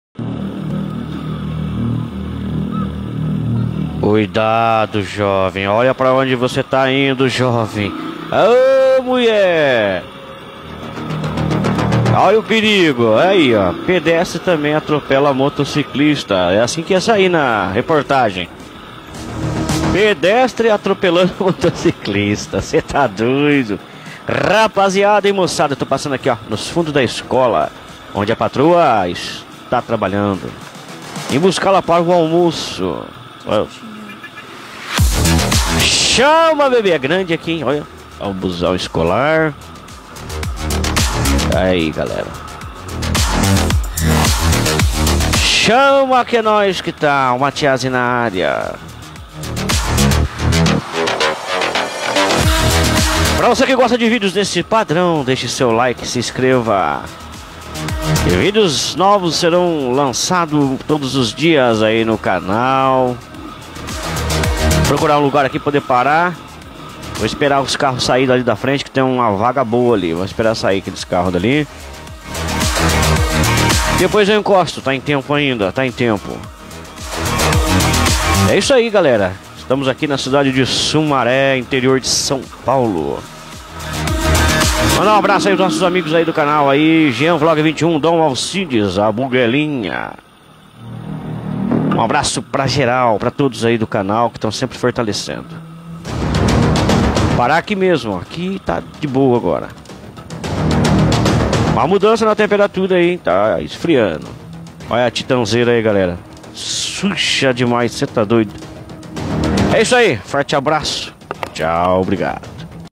FATOS INUSITADOS NO TRÂNSITO
SITUAÇÕES INUSITADAS NO TRÂNSITO DENTRO DA CIDADE, SUSTOS RISCOS DO COTIDIANO EM DUAS RODAS E MANUTENÇÕES DA MOTO
CANAIS PARCEIROS:

Câmera xiaomi yi4k
equipamentos: live the sound card
headset A-302 Altomex